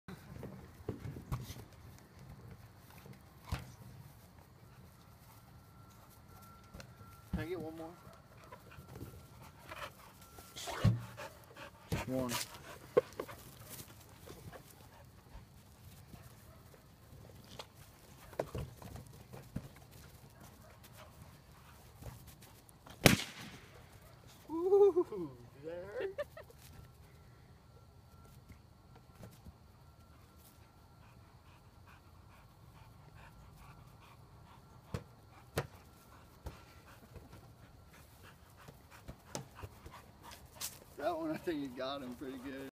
Can't touch the ground
Tank the bulldog must keep the balloon in the air to win.